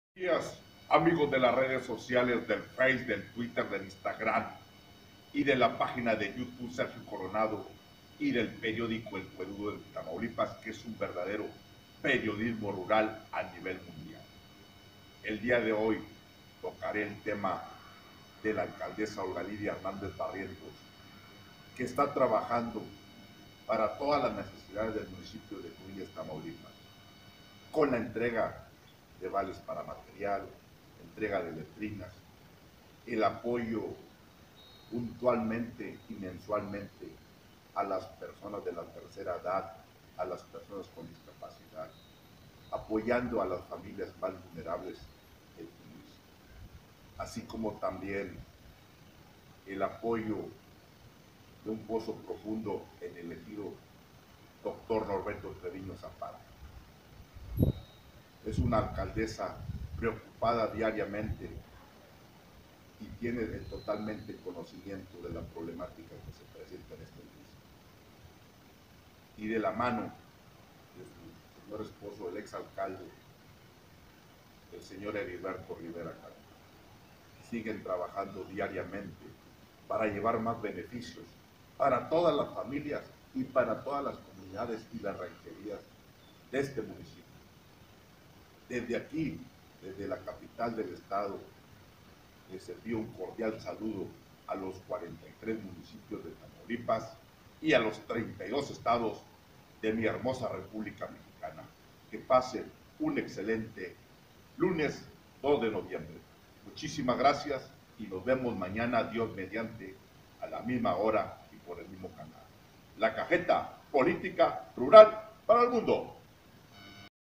En el Punto de Vista de La Cajeta Política Rural Mundial Ing. Sergio Coronado
En el Punto de Vista de La Cajeta Política Rural Mundial Ing. Sergio Coronado Apoyando a todas las familias más vulnerables del Mpio así como el apoyo mensual a las personas de la tercera edad y con discapacidad entrega de vales de material ; el apoyo con un pozo profundo en Ejido Dr. Norberto Treviño Zapata Olga Lidia Hernández Barrientos alcaldesa de Cruillas Tamaulipas .